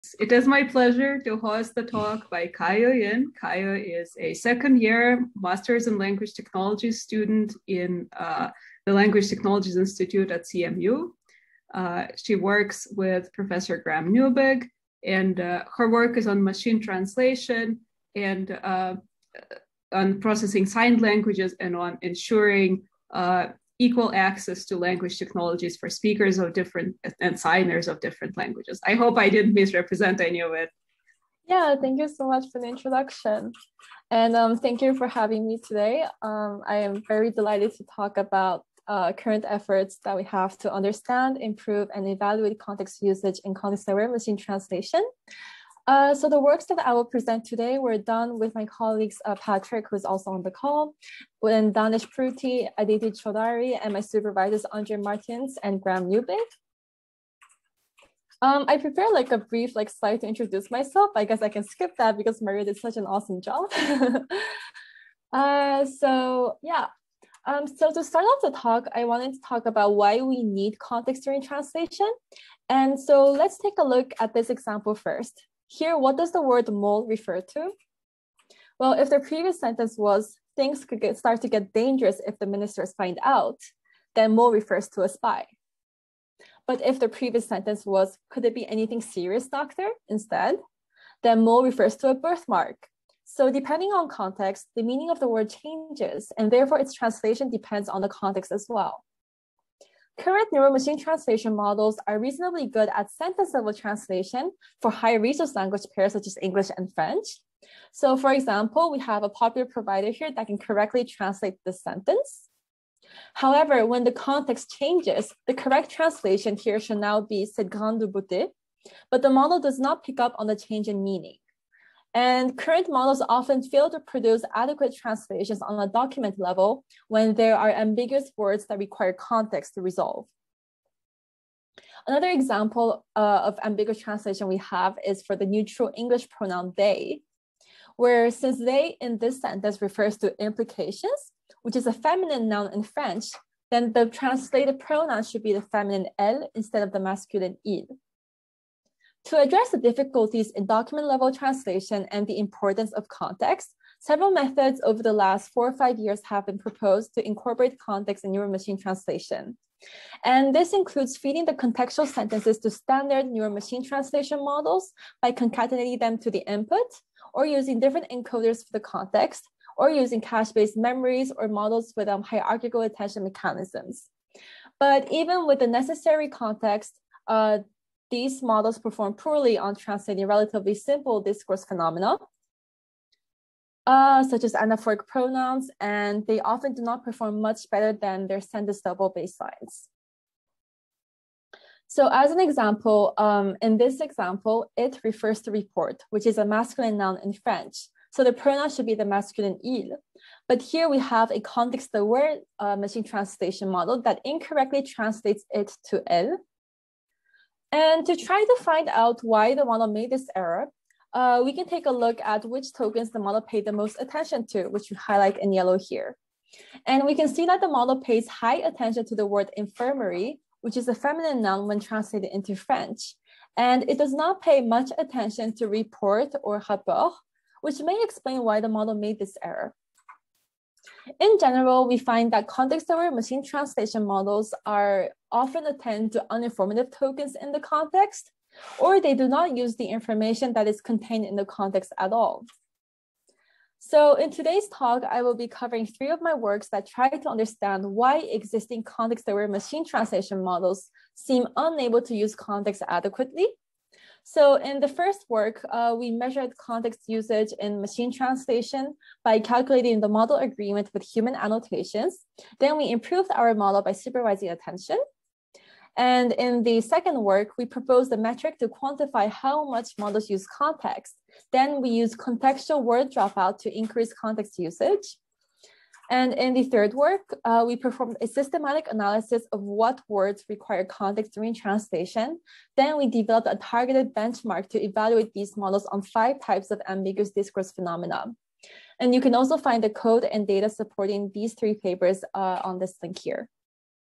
SIGTYP Lecture Series: Kayo Yin. Understanding Context Usage in Context-aware Machine Translation.P1
SIGTYP Lecture Series: Kayo Yin.  Understanding, Improving and Evaluating Context Usage in Context-aware Machine Translation (Part 1)

Context-aware Neural Machine Translation (NMT) models have been proposed to perform document-level translation, where certain words require information from the previous sentences to be translated accurately. However, these models are unable to use context adequately and often fail to translate relatively simple discourse phenomena. In this talk, I will discuss methods to measure context usage in NMT by using human annotations and conditional cross mutual information, as well as training methods to improve context usage by supervising attention and performing contextual word dropout. I will also discuss ways to identify words that require context to translate and how to evaluate NMT models on these ambiguous phenomena, and present open challenges in document-level translation.